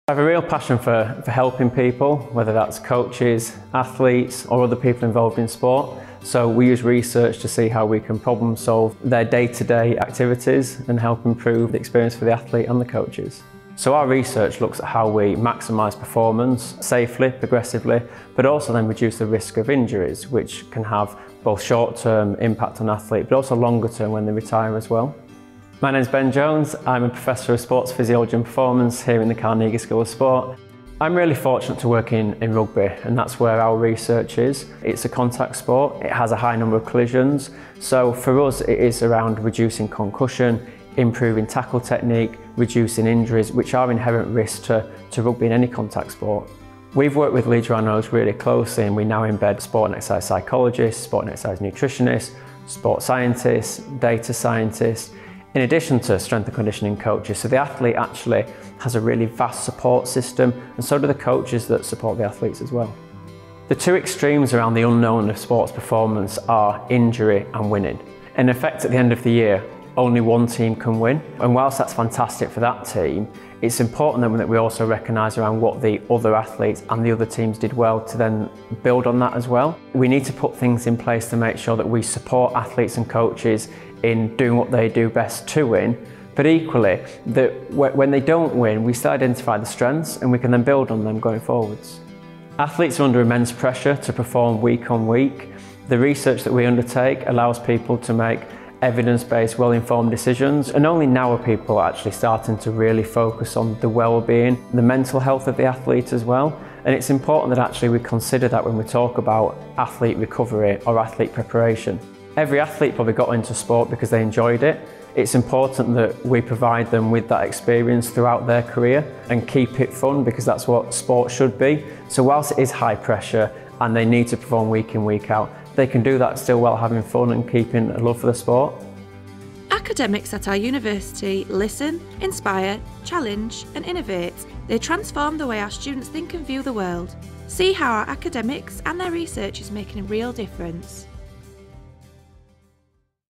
Transforming how we support athletes.
Professor Ben Jones works in Sports Physiology and Performance. He’s interested in helping to reduce injuries to athletes and supporting them and their coaches by identifying their strengths and building on them. He also looks at the mental health and wellbeing of athletes and aims to help them keep their sport fun while still performing well.